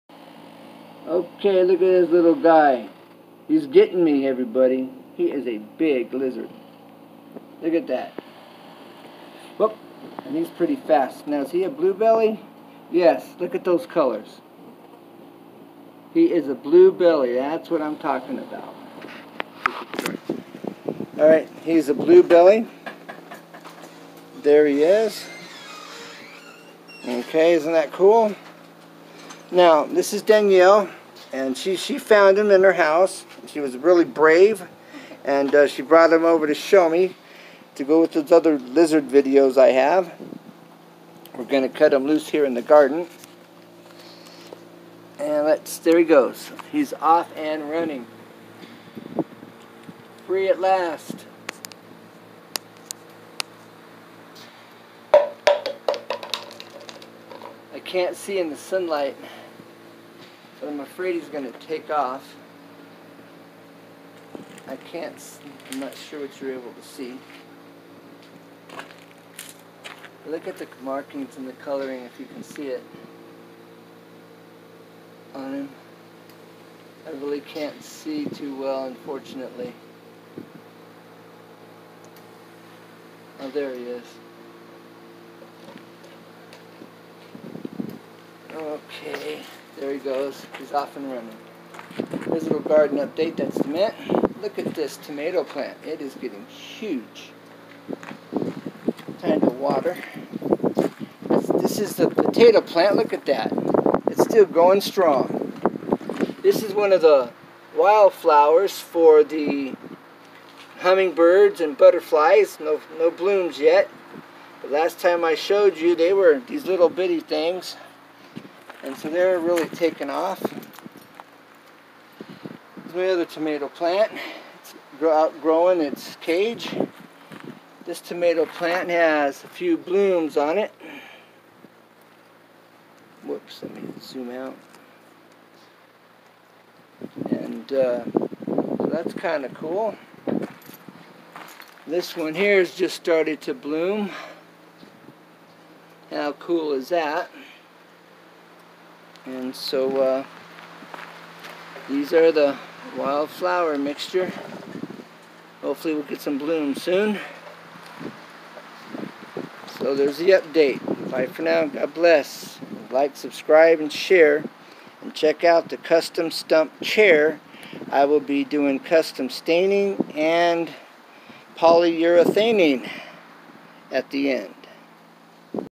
Blue belly lizard and garden
Blue belly lizard and garden come on in and check out this blue belly lizard he's a biggin forward to hear from you LightScribe and share in common pleas